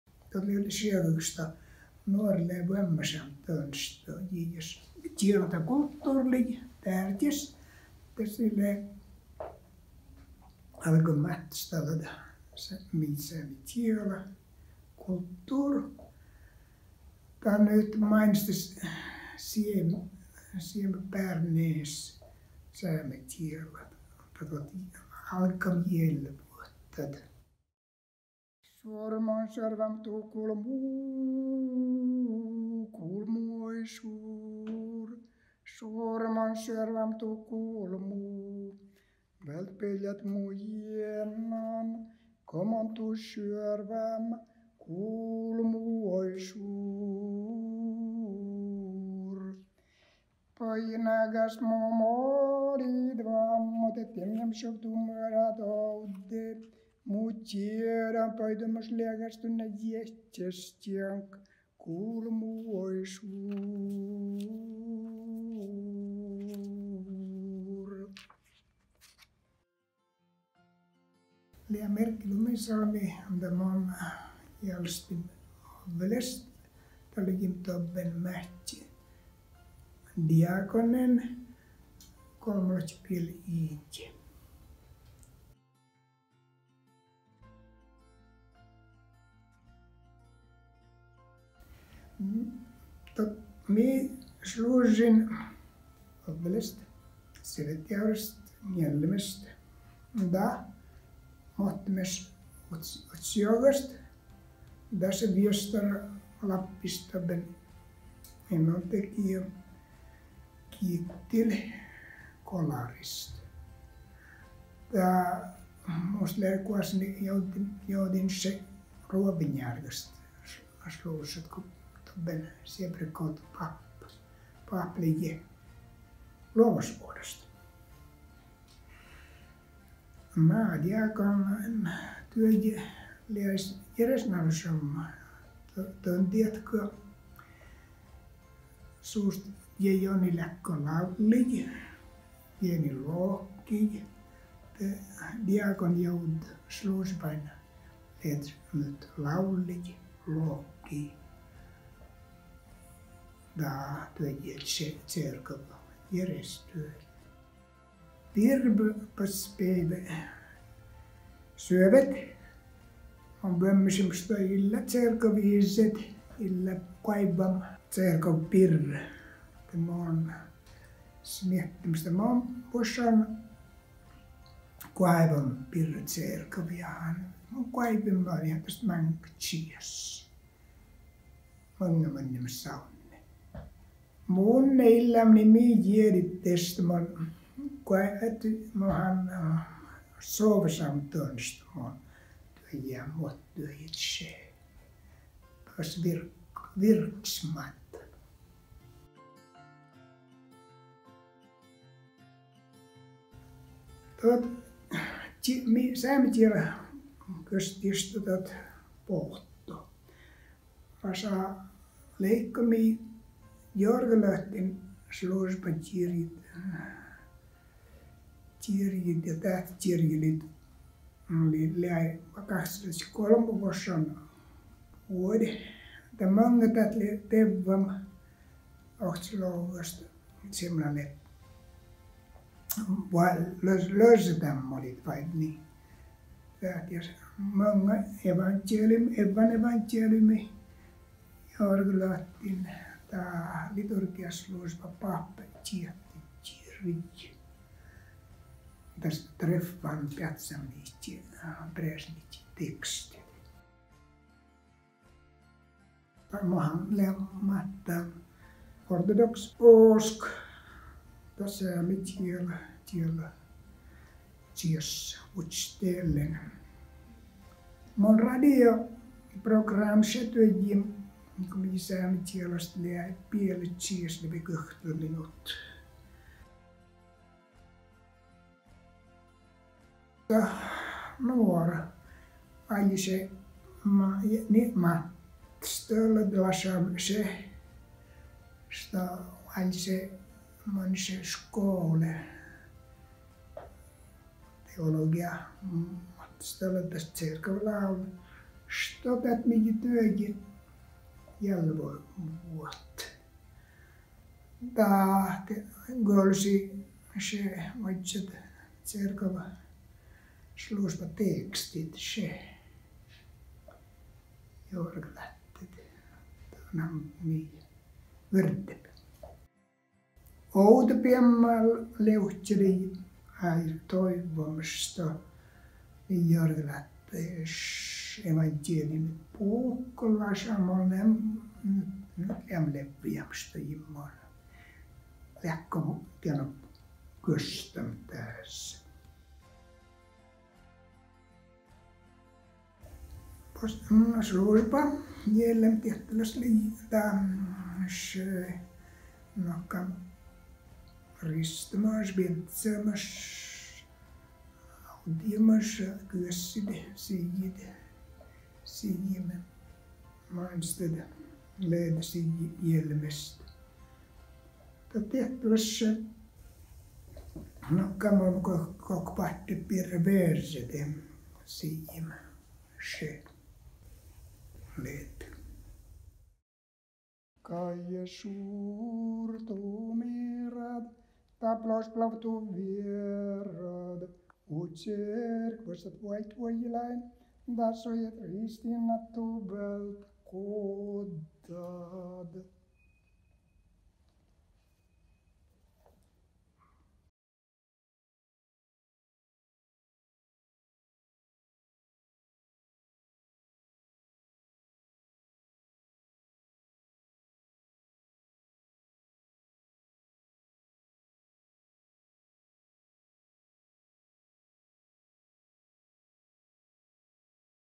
Kirkko toimii: kirkon työssä – diakoni ja kanttori Erkki Lumisalmi, Ivalo
Saamelaisten kansallispäivänä kuulemme, kuinka diakoni Erkki Lumisalmi kertoo koltansaameksi kirkon työstä Suomen pohjoisimmassa seurakunnassa.